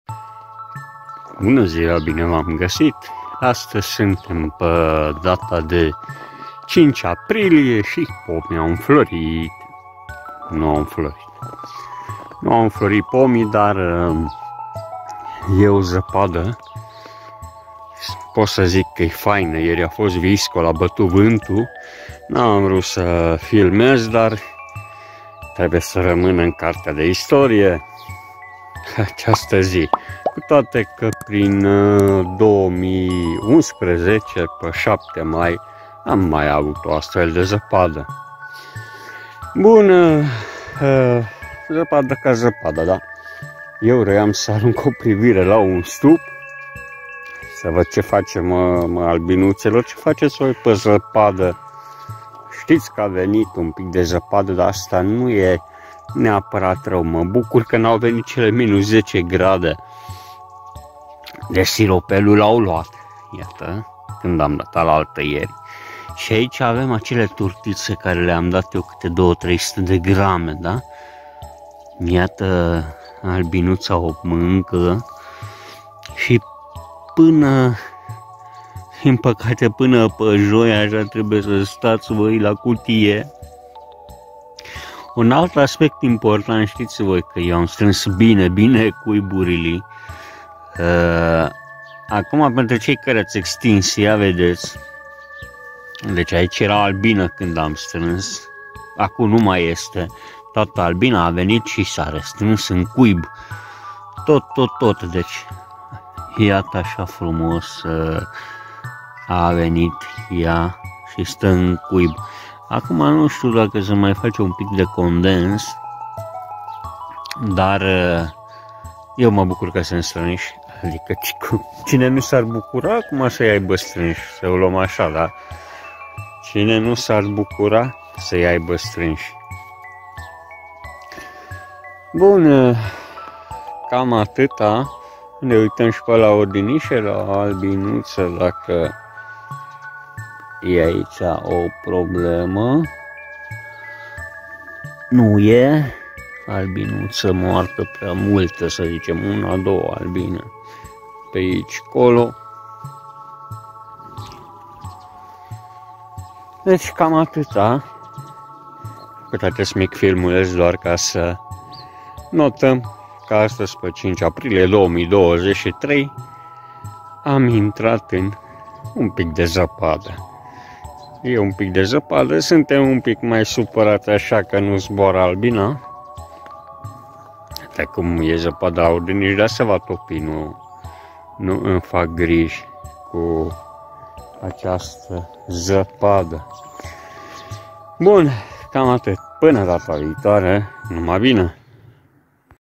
A venit zapada , Ce fac albinele pe aceasta vreme ? Sa aruncam o privire
A venit iarna , dar este doar o rabufnire , zapada e bine venita , mai bine zis umiditatea din sol e bine venita . Sa arunc o privire la albinute sa vedem ce fac pe aceasta vreme .